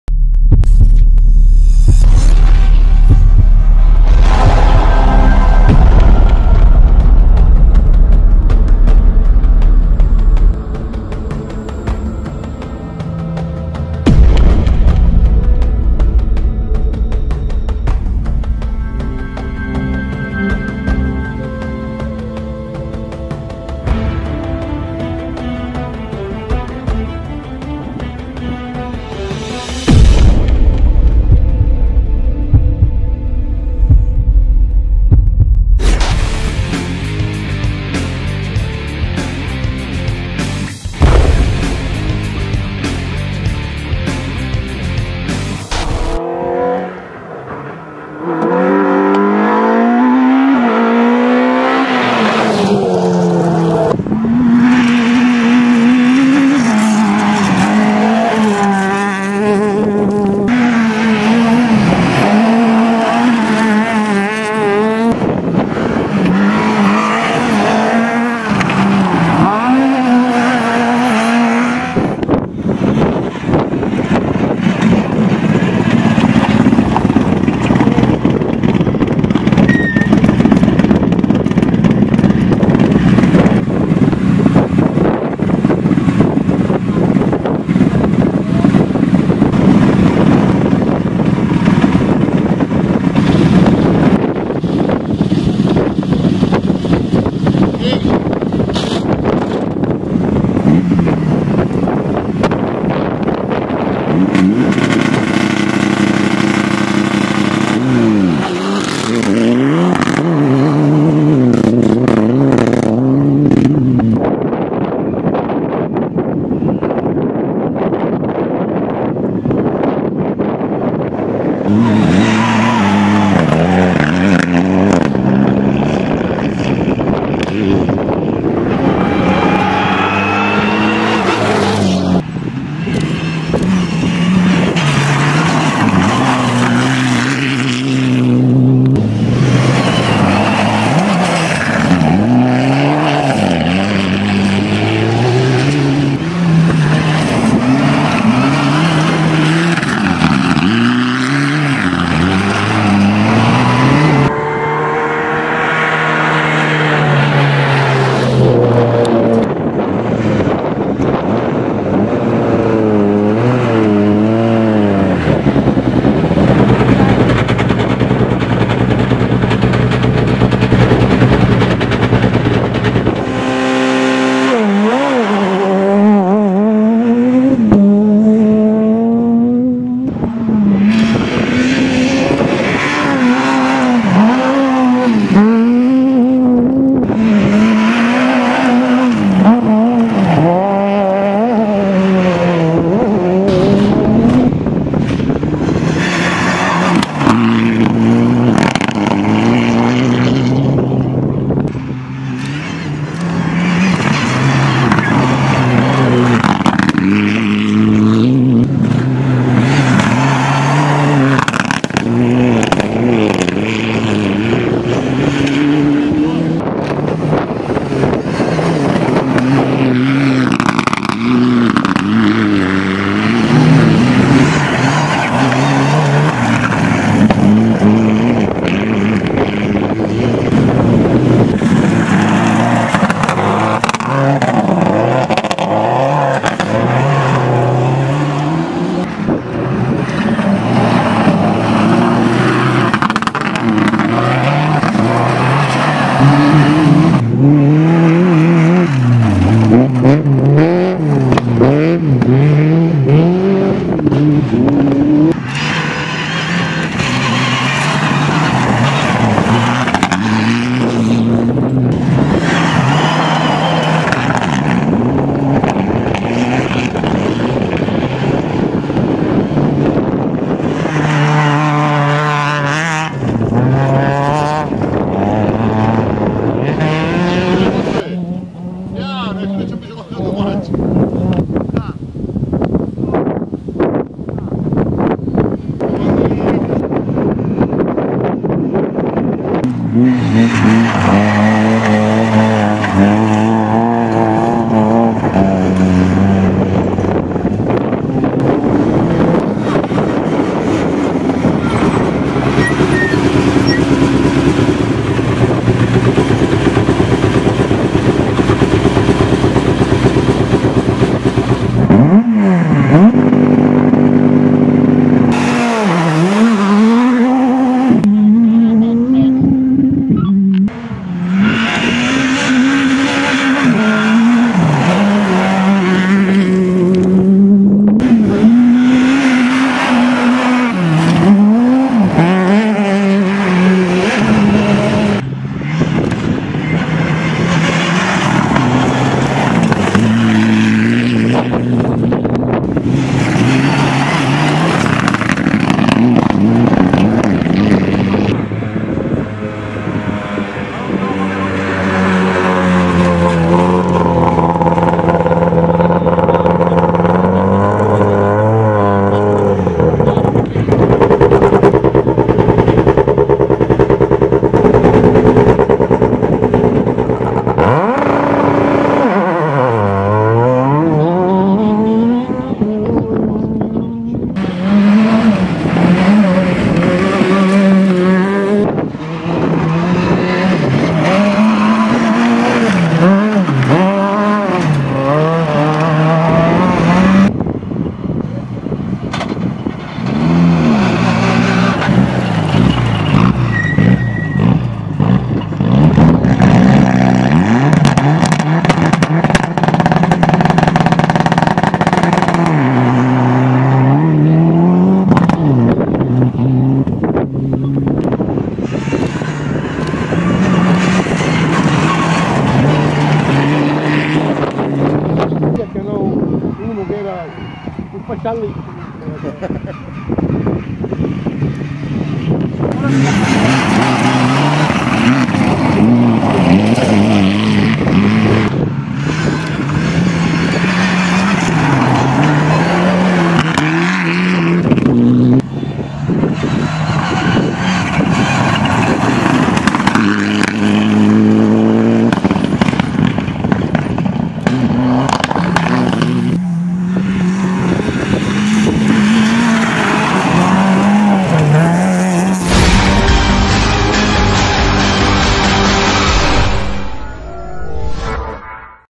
Shakedown 33° Rally Conca d'Oro © SICILIARALLY.COM
Le prime immagini integrali dello Shakedown del 33° Rally Conca d'Oro © Una produzione di Peppe Natoli per - info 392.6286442